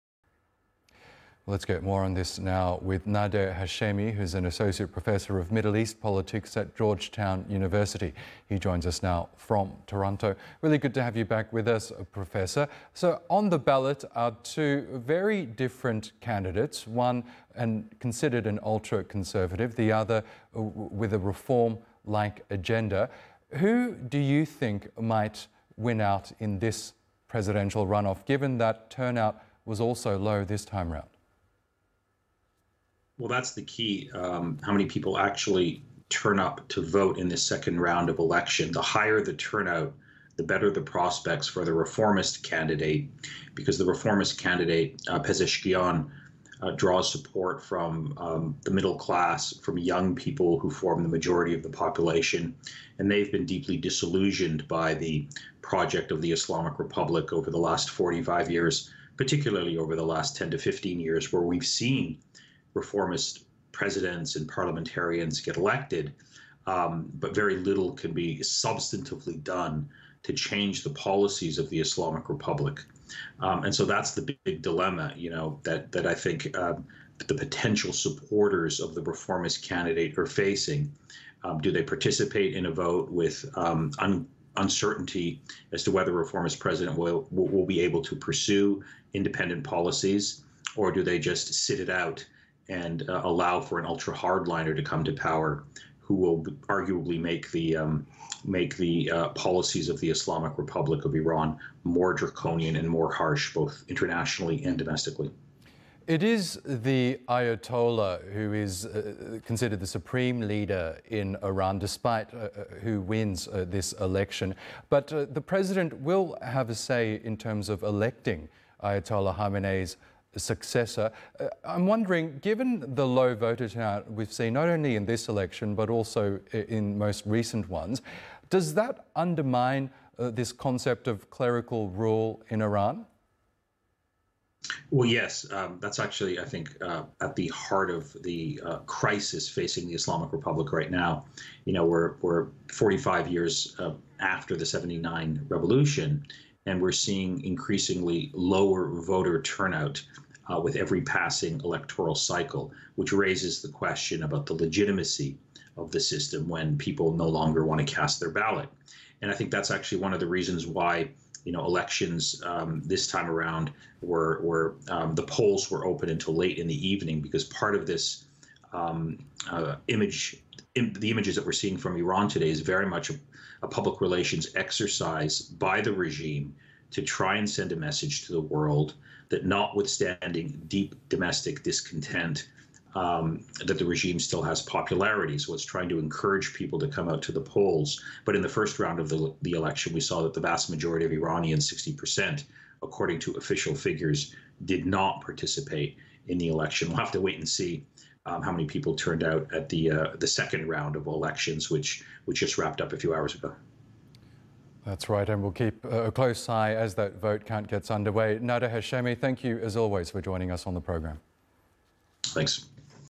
Nader Hashemi on 2024 Iranian Presidential Election
Analysis of electoral authoritarianism in Iran